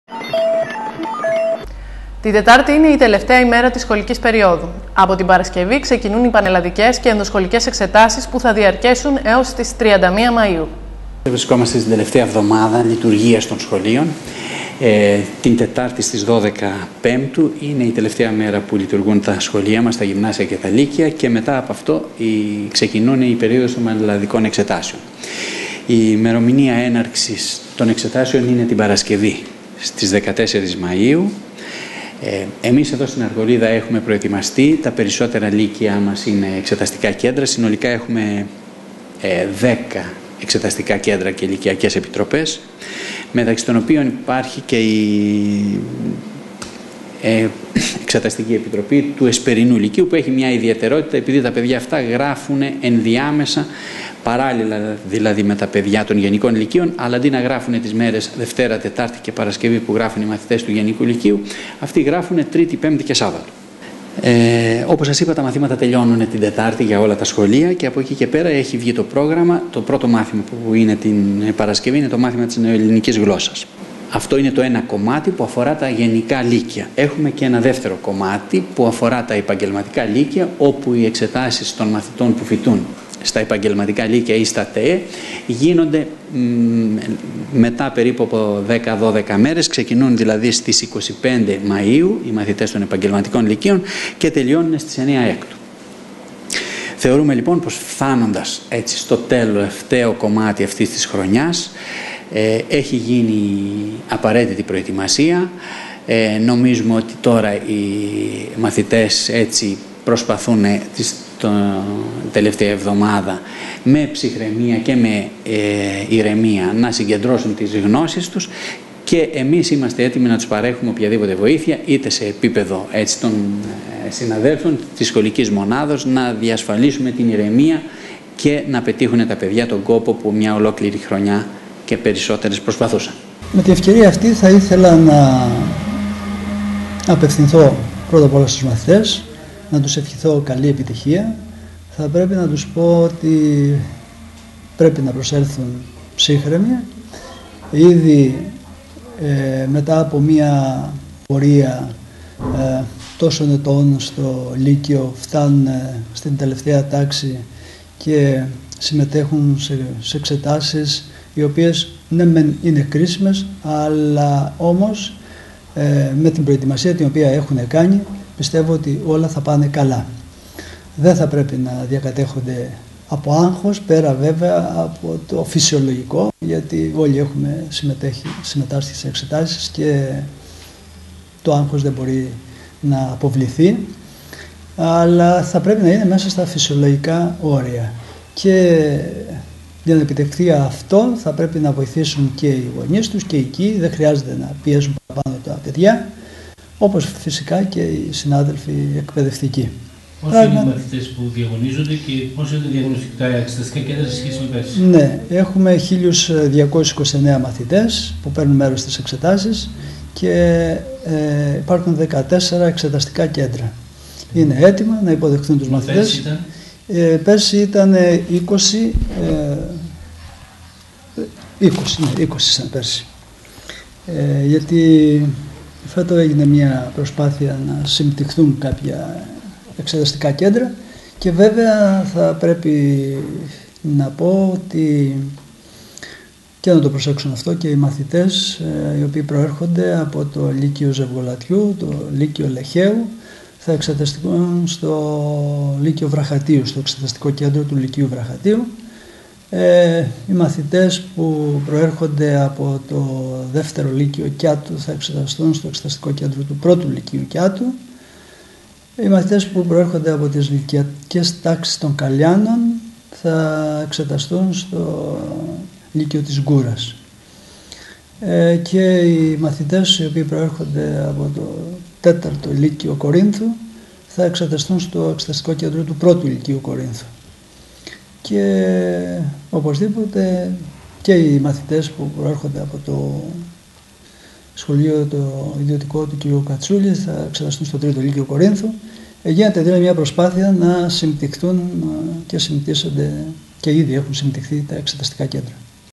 AΡΓΟΛΙΔΑ / ΚΟΡΙΝΘΟΣ 11 5 2010 DRTV
Εκπαίδευση - Ξεκινούν οι πανελλαδικές εξετάσεις.